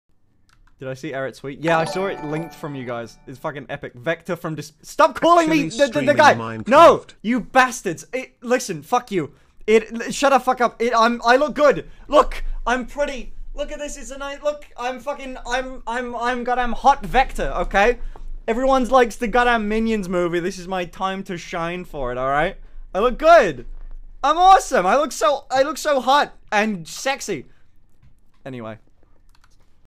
F1nn's fit for MCC 23
This is a clip from F1nn5ter's Twitch stream entitled MCC! - CARRYING MY ENTIRE TEAM ON MY BACK which originally streamed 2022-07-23.

This is a fan-made and uploaded video. F1nn5ter retains all rights to the stream content.